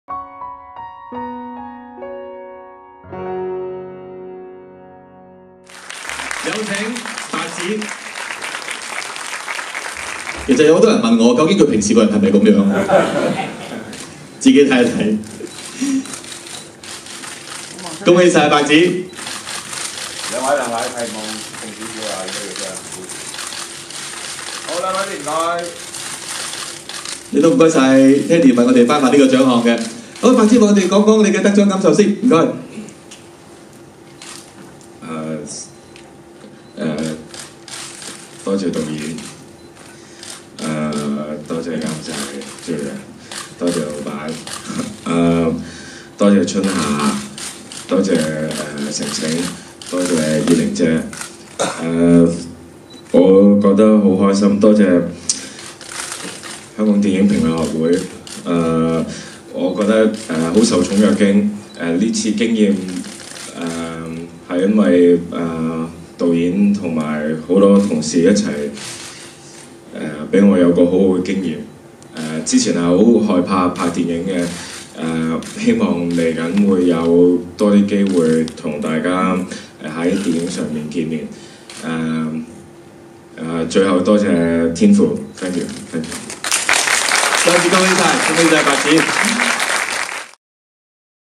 第二十二屆香港電影評論學會大獎頒獎禮──最佳男演員：白只（《踏血尋梅》）
得獎者白只先生發表得獎感言。